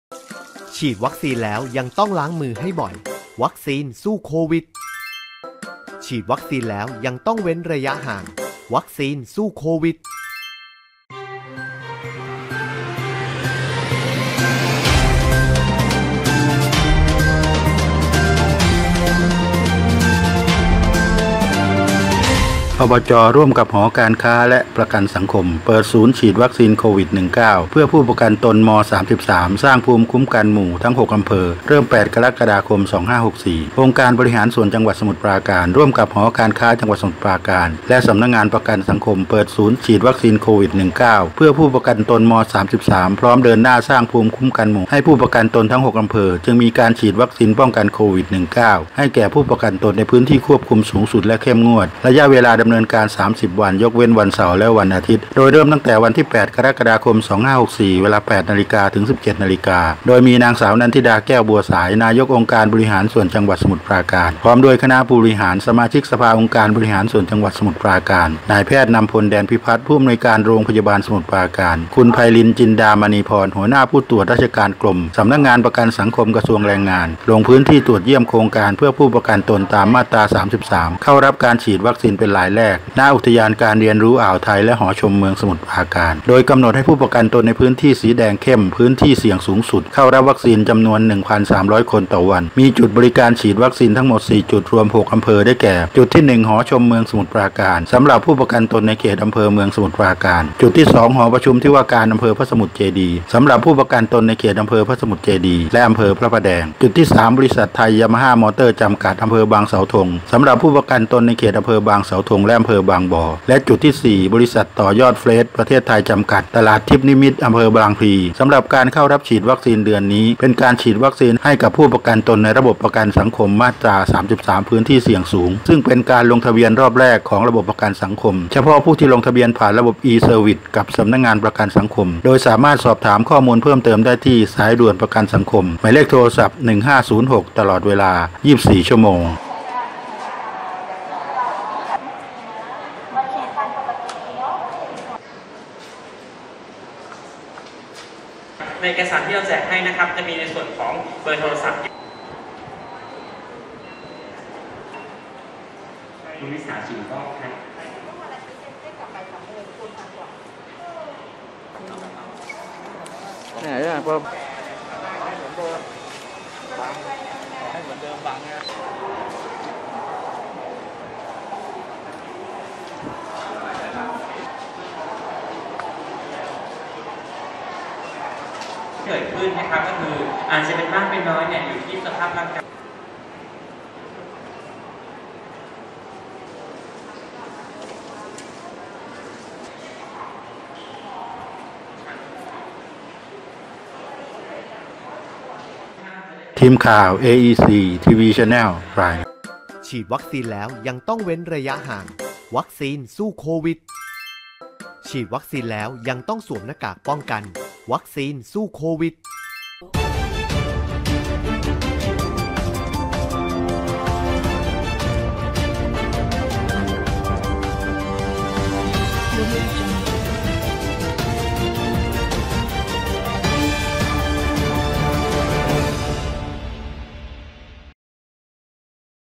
อบจ ร่วมกับ หอการค้าฯ และประกันสังคม เปิดศูนย์ฉีดวัคซีนโควิด 19 เพื่อผู้ประกันตน ม 33 สร้างภูมิคุ้ม
อบจ. ร่วมกับ หอการค้าฯ และประกันสังคม เปิดศูนย์ฉีดวัคซีนโควิด-19 เพื่อผู้ประกันตน ม.33 สร้างภูมิคุ้มกันหมู่ทั้ง 6 อำเภอ เริ่ม 8 กรกฎาคม 2564 tvonline​​​​​​​​​​​​​​​​​​​​​​​​​​​​​​​​​​​​​​​​​​​​​​​​​​​​​​​​​​​​​​​ tvstreaming​​​​​​​​​​​​​​​​​​​​​​​​​​​​​​​​​​​​​​​​​​​​​​​​​​​​​​​​​​​​​​​ #

ดูทีวีสดผ่านมือถือที่นี่ #
        เมื่อเวลา 10.00 น.วันที่ 8 ก.ค.64 องค์การบริหารส่วนจังหวัดสมุทรปราการ ร่วมกับ หอการค้าจังหวัดสมุทรปราการ และสำนักงานประกันสังคม เปิดศูนย์ฉีดวัคซีนโควิด-19 เพื่อผู้ประกันตน ม.33 พร้อมเดินหน้าสร้างภูมิคุ้มกันหมู่ให้ผู้ประกันตน ทั้ง 6 อำเภอ จึงได้มีการฉีดวัคซีนป้องกันโควิด-19 ให้แก่ผู้ประกันตน ในพื้นที่ควบคุมสูงสุดและเข้มงวด ระยะเวลาดำเนินการ 30 วัน ยกเว้นวันเสาร์ และวันอาทิตย์ โดยเริ่มตั้งแต่ วันที่ 8 กรกฎาคม 2564 เวลา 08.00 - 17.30 น. โดยมี น.ส.นันทิดา แก้วบัวสาย นายกองค์การบริหารส่วนจังหวัดสมุทรปราการ พร้อมด้วย คณะผู้บริหาร สมาชิกสภาองค์การบริหารส่วนจังหวัดสมุทรปราการ นายแพทย์นำพล แดนพิพัฒน์ ผู้อำนวยการโรงพยาบาลสมุทรปราการ คุณไพลิน จินดามณีพร หัวหน้าผู้ตรวจราชการกรม สำนักงานประกันสังคม กระทรวงแรงงาน ลงพื้นที่ตรวจเยี่ยมโครงการเพื่อผู้ประกันตนตามมาตรา 33 เข้ารับการฉีดวัคซีนเป็นวันแรก ณ อุทยานการเรียนรู้อ่าวไทย และหอชมเมืองสมุทรปราการ
         โดยกำหนดให้ผู้ประกันตนในพื้นที่สีแดงเข้ม (พื้นที่เสี่ยงสูงสุด) เข้ารับวัคซีน จำนวน 1,300 คน ต่อวัน มีจุดบริการฉีดวัคซีนทั้งหมด 4 จุดรวม 6 อำเภอ ได้แก่ จุดที่ 1 หอชมเมืองสมุทรปราการ สำหรับผู้ประกันตนในเขตอำเภอเมือง สมุทรปราการ จุดที่ 2 หอประชุมที่ว่าการอำเภอพระสมุทรเจดีย์ สำหรับผู้ประกันตนในเขต อำเภอพระสมุทรเจดีย์ และอำเภอพระประแดง จุดที่ 3 บริษัท ไทยยามาฮ่ามอเตอร์ จำกัด อำเภอบางเสาธง สำหรับผู้ประกันตนในเขตอำเภอบางเสาธง และอำเภอบางบ่อ และจุดที่ 4 บริษัท ต่อยอดเฟรช (ประเทศไทย) จำกัด (ตลาดทิพย์นิมิตร) อำเภอบางพลี สำหรับการเข้ารับวัคซีนเดือนนี้ เป็นการฉีดวัคซีนให้กับผู้ประกันตน ในระบบประกันสังคมมาตรา 33 พื้นที่เสี่ยงสูง ซึ่งเป็นการลงทะเบียนรอบแรกของระบบประกันสังคม เฉพาะผู้ที่ลงทะเบียนผ่านระบบ e-service กับ สำนักงานประกันสังคม โดยสามารถสอบถามข้อมูลเพิ่มเติมได้ที่ สายด่วนประกันสังคม โทร 1506 ตลอด 24 ชั่วโมง